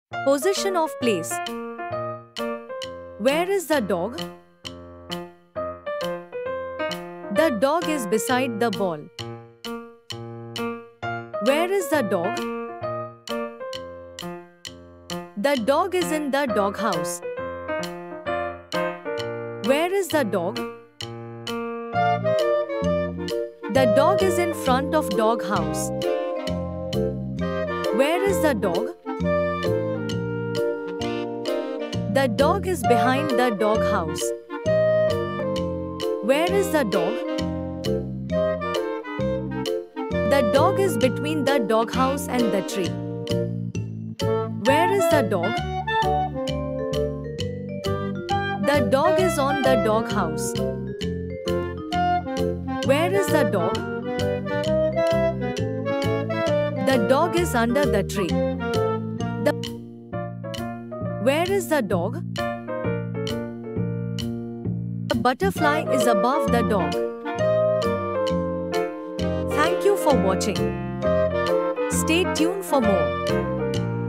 Where is the Dog? Learn Positions of Place in English | Bttvkids
Welcome to our fun and interactive lesson on positions of place! In this video, we’ll help you understand spatial relationships using the example of “Where is the dog?”

Through engaging visuals and relatable scenarios, you’ll learn essential phrases and vocabulary related to positions such as "in," "on," "under," "next to," "behind," and more. We’ll show you a friendly dog placed in various locations around a home, allowing you to practice identifying where the dog is located in relation to other objects.

This lesson is perfect for kids, language learners, or anyone looking to improve their understanding of English spatial terms. Join us as we ask questions like, “Where is the dog?” and guide you to the answers. You’ll get to see the dog placed in different positions, helping reinforce the concepts as you learn.

To make it even more engaging, we encourage viewers to participate by answering questions in the comments section. Let’s explore the world of positions together!